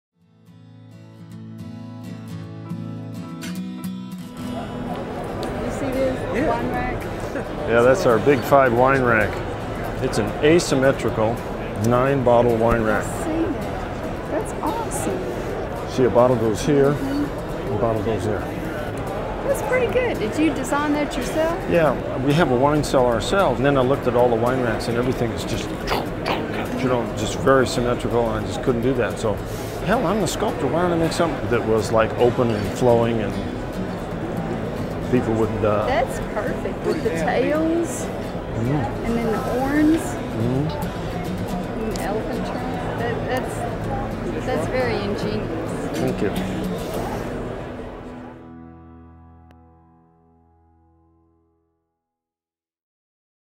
Big 5 Wine Rack
Carlsbad, California based artist Mark Nordquist shows his sculpture at the SCI show in Reno, Nevada.  See more of Mark's sculptures, chess sets, wine racks and wine bottle stoppers @ marknordquiststudio.com